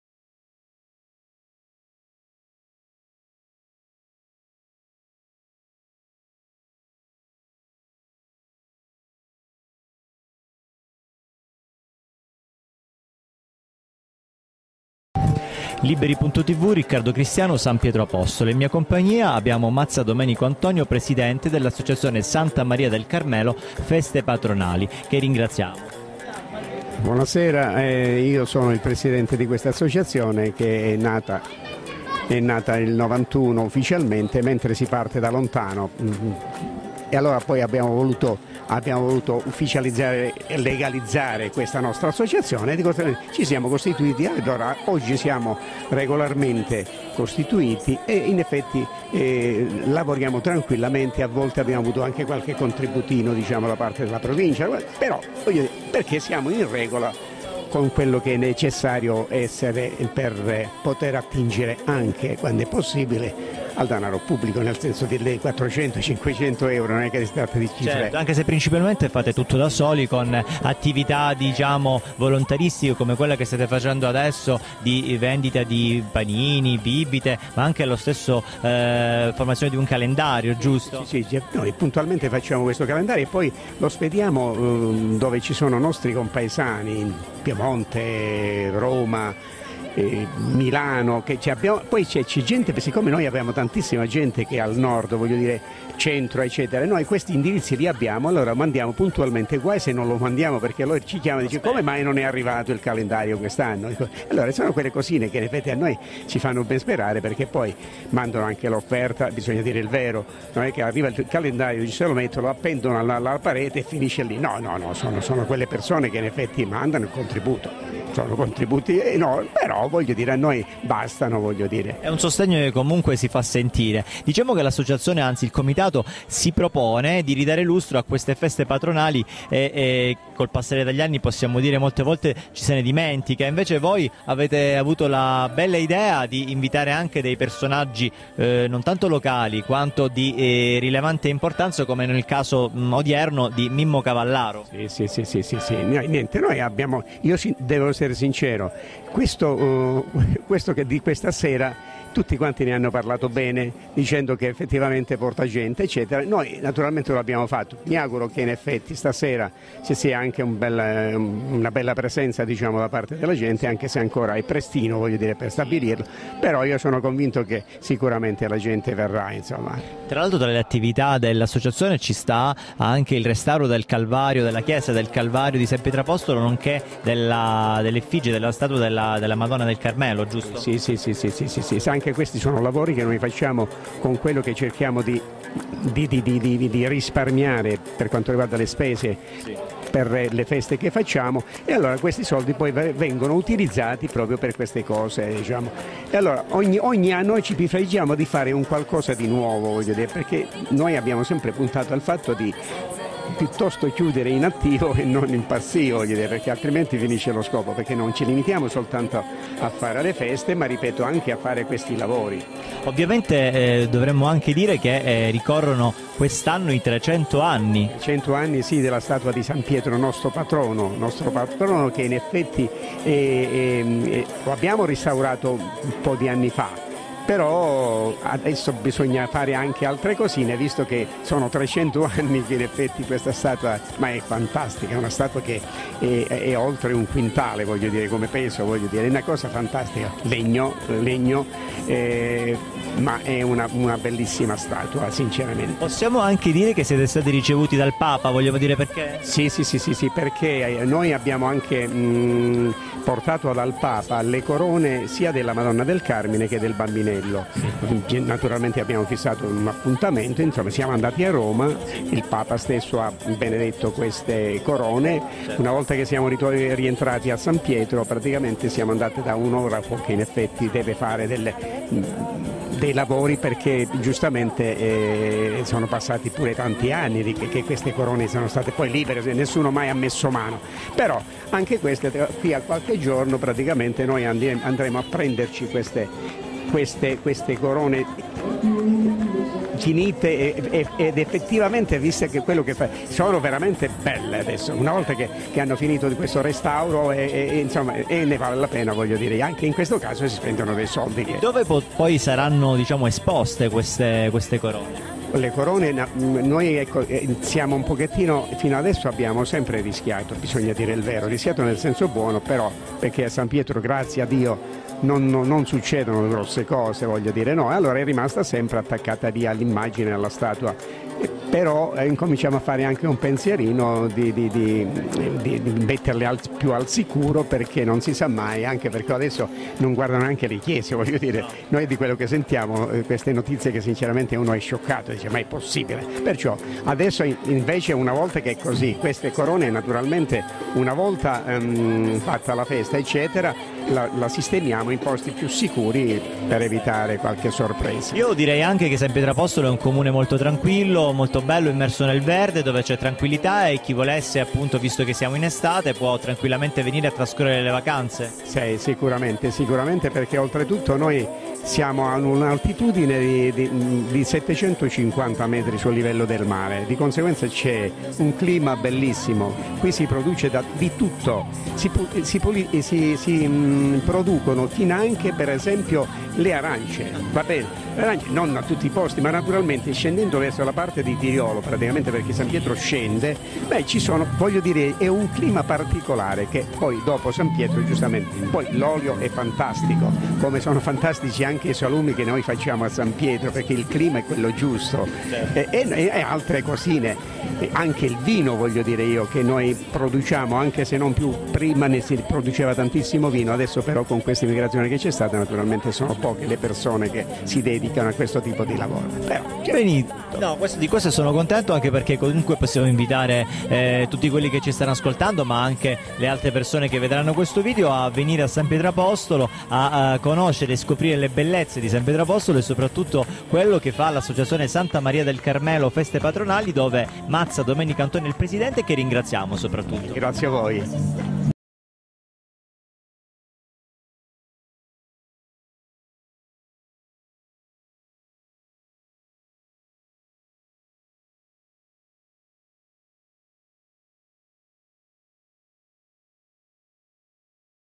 Intervista a Mazza Domenico Antonio, Presidente Ass.ne S. Maria del Carmelo "Feste Patronali" (CZ)
Intervista a Mazza Domenico Antonio, Presidente Associazione S.S. Maria del Carmelo - "Feste Patronali"

Incontro in occasione del concerto di Mimmo Cavallaro ) & Cosimo Papandrea.
Festa San Pietro Apostolo, Catanzaro 28 giugno 2014.
Associazione S.S. Maria del Carmelo - "Feste Patronali" (Catanzaro).